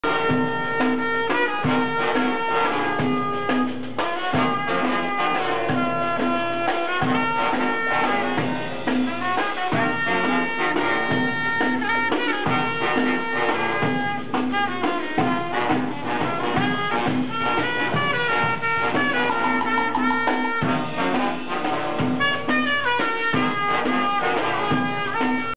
Subway Horn Section
A 10 piece brass band plays underground in a mid town subway, New York City.